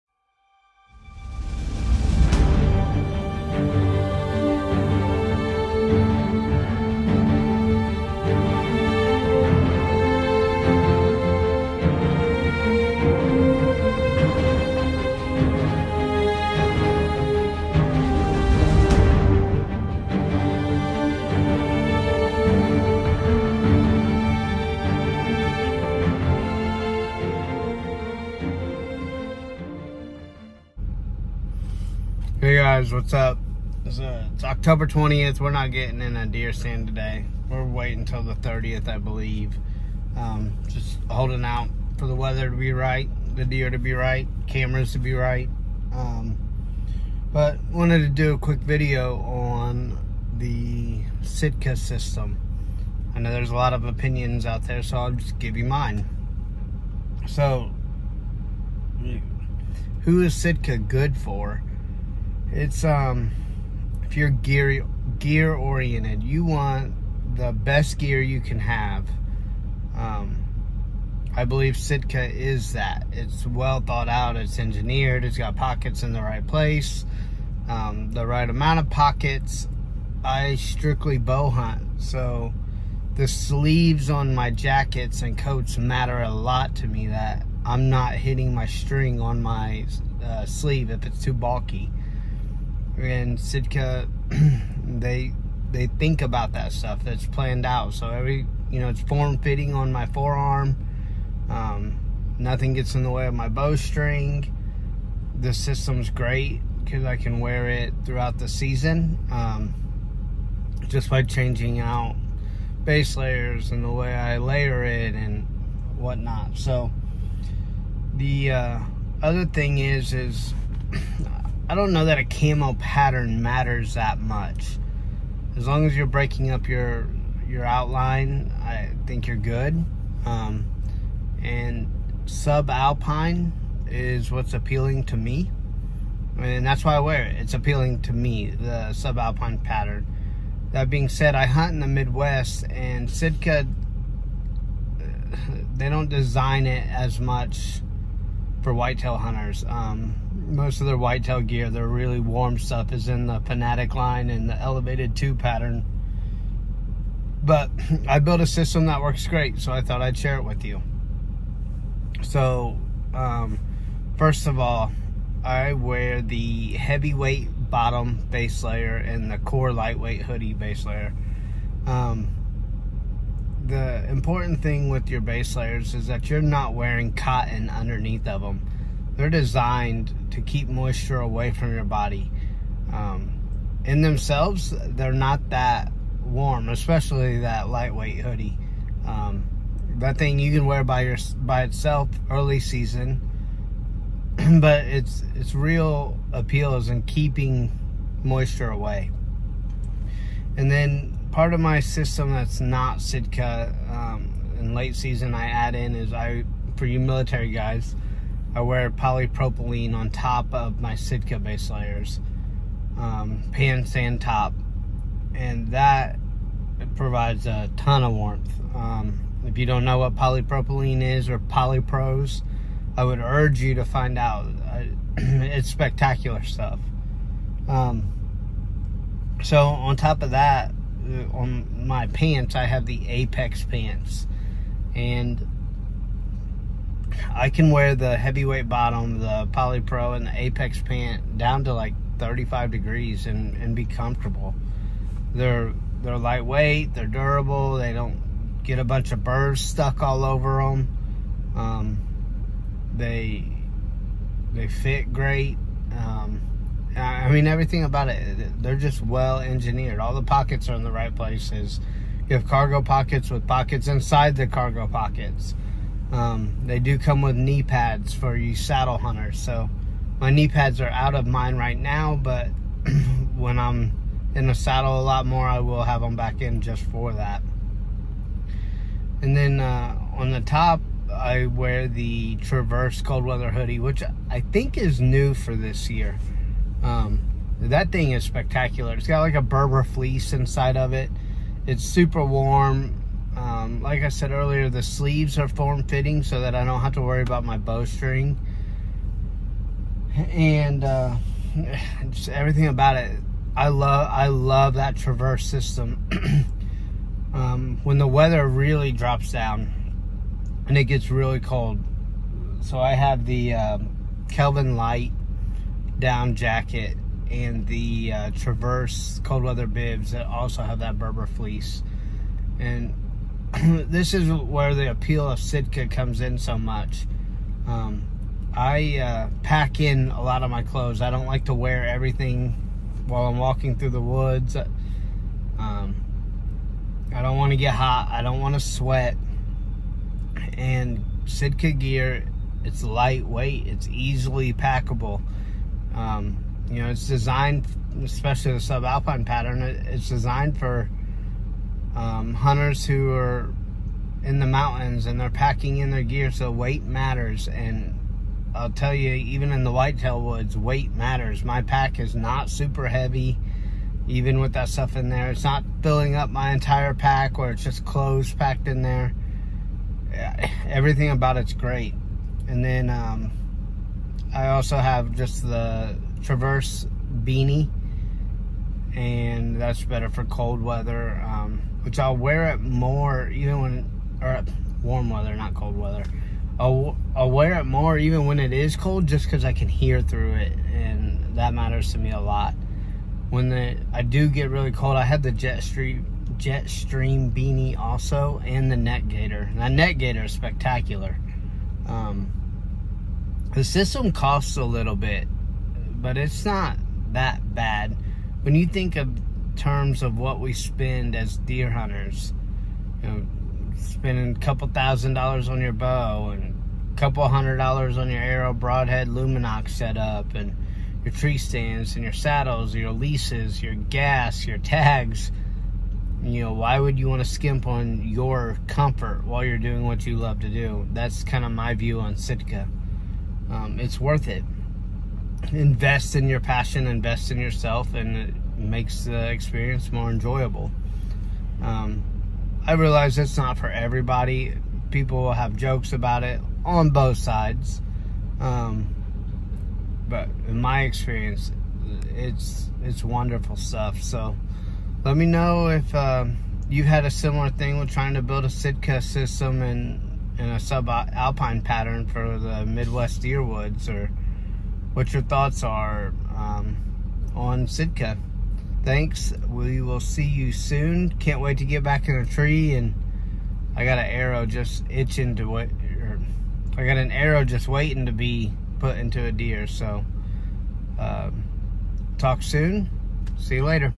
My Subalpine Sitka system for Whitetails
This week, we are talking about a Sitka Subalpine system for the Midwest! Let us know what you think!

Sitka Heavyweight Bottom Subalpine
Sitka Core Lightweight Hoodie Subalpine
Sitka Apex Pants Subalpine
Sitka Traverse Cold Weather Hoodie Subalpine
Sitka Traverse Bibs Subalpine
Sitka Kelvin Lite Jacket Subalpine
Sitka Traverse Beanie Subalpine
Sitka Jetstream Beanie Subalpine
Sitka Neck Gaiter Subalpine

Any and all support is so greatly appreciated!